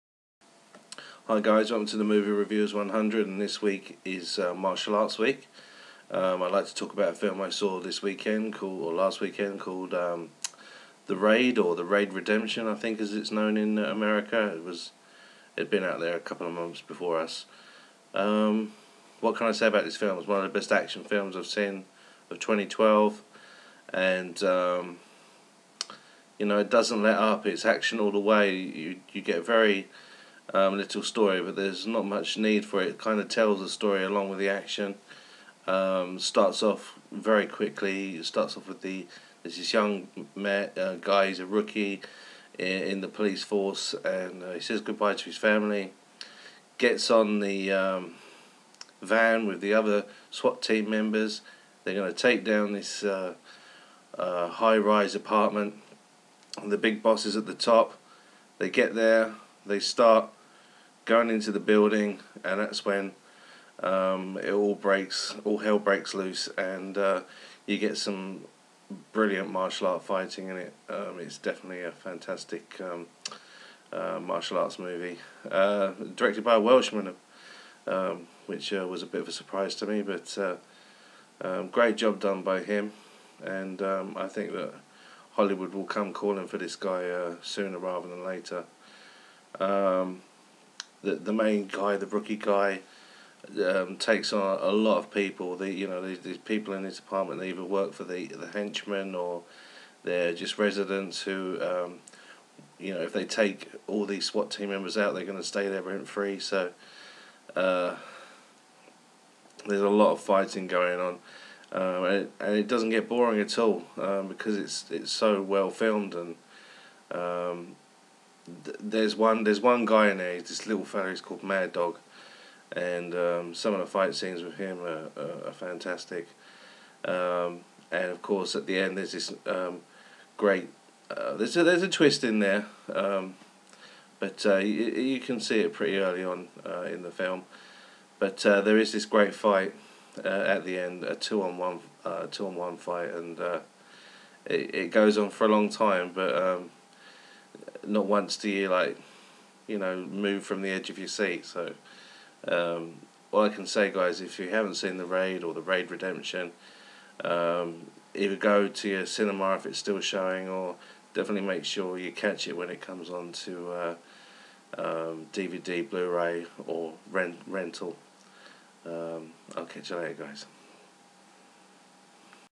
The Raid movie review / Kung Fu week
Here is my review of the Raid, or the Raid Redemption as it is know in America